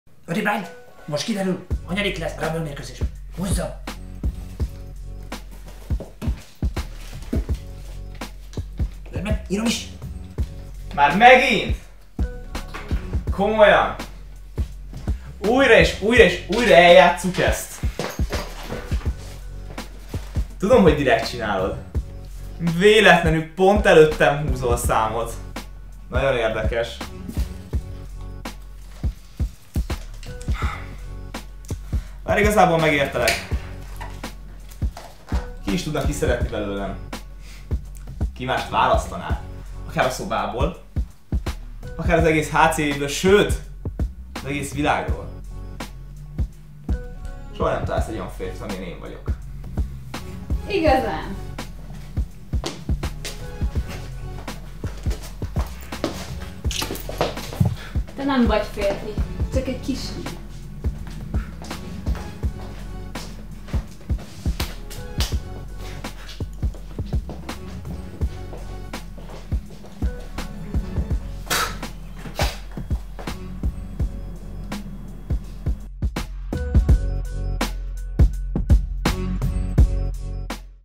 Audrey Bride száma
Audrey Bride is megérkezik, hogy kihúzza az érkezési számát a Rumble mérkőzéshez, ám megjelenik Dylan.